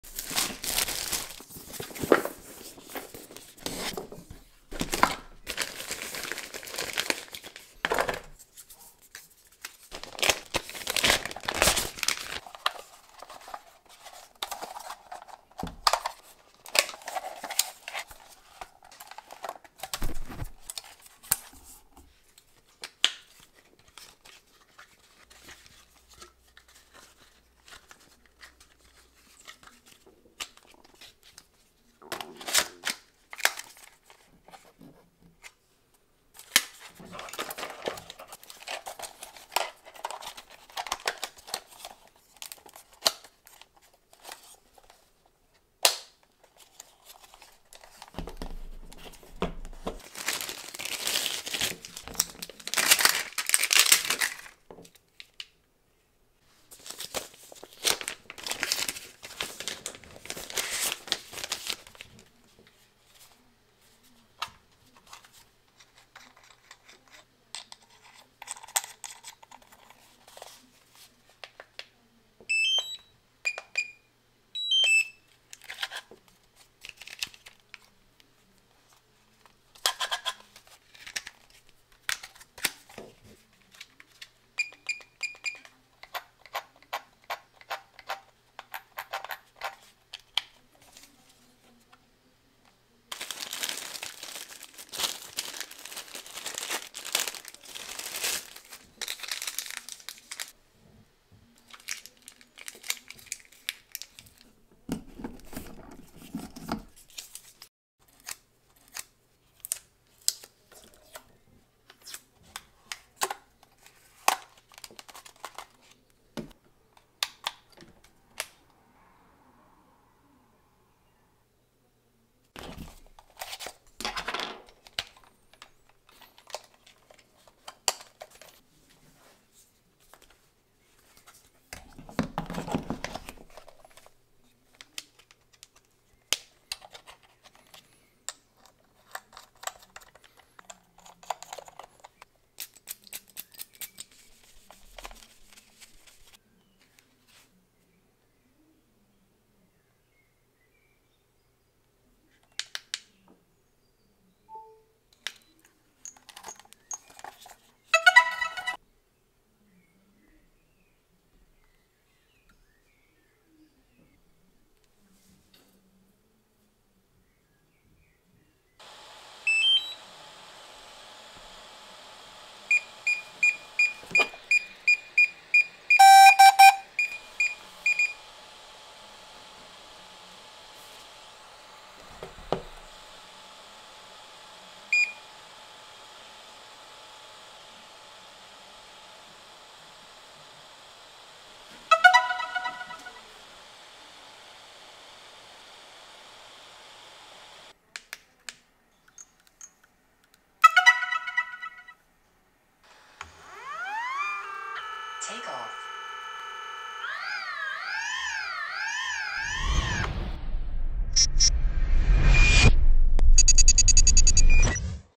DJI Mavic 3 - ROZPAKOWANIE + PIERWSZE URUCHOMIENIE - ASMR
Czekałem pół roku od premiery DJI Mavic 3 i cały czas wahałem się z zakupem.
Ten model nie osiągnął pełnej funkcjonalności, były problemy z GPS, użytkownicy nie byli zadowoleni, sporo pojawiało się zwrotów.
Dopiero ostatnie dwa kroki, jakie wykonała firma DJI, pozwoliły mi na podjęcie decyzji o zakupie mojego najdroższego drona jak dotąd.
Pierwszy krok to promocja na stronie dji.com a drugi to aktualizacja oprogramowania.

🕑 TIMESTAMPS:

0:00 Rozpakowywanie
0:50 Ładowarka
0:56 Aparatura DJI RC-N1
1:37 Zapasowe drążki  + kable RC,
1:44 Demontaż śmigła
1:47 Usuwanie naklejek
1:54 Szybkie ładowanie
2:02 Kontroler + tablet
2:26 Aplikacja DJI Fly + aktualizacje
2:50 Linkowanie (połączenie drona z pilotem)
3:00 Aktywacja
3:05 Aktualizacja oprogramowania drona
3:16 Gotowy do lotu